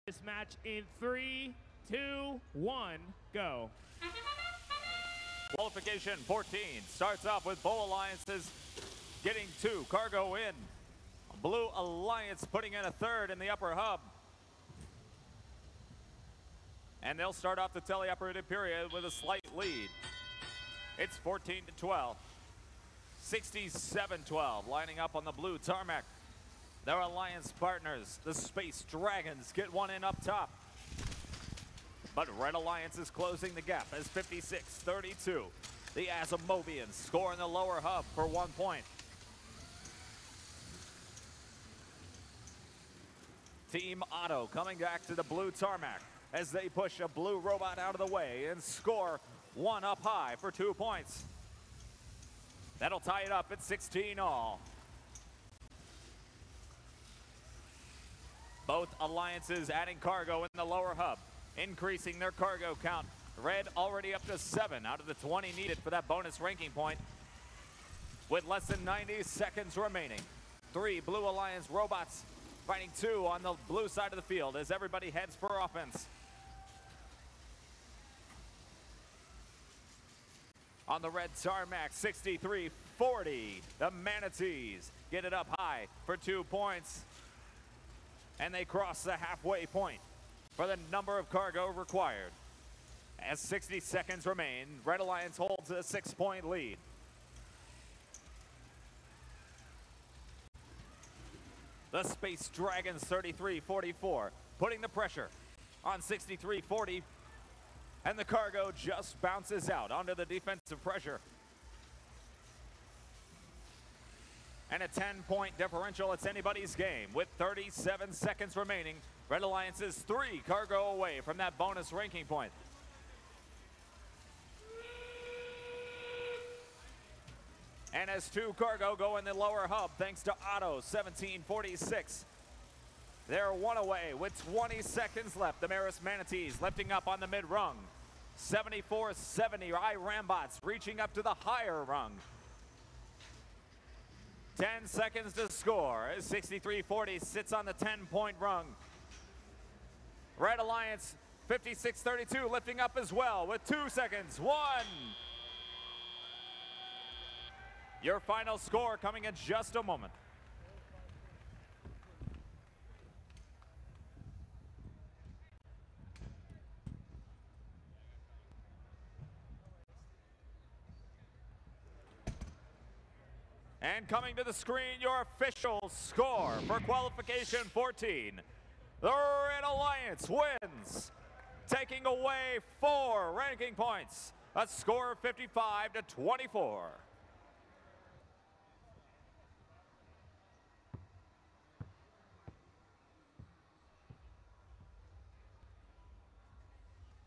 PCH District State Championship - Qualification Match 14
Footage of the PCH District State Championship is courtesy of the GeorgiaFIRST A/V Team.

To view match schedules and results for this event, visit The Blue Alliance Event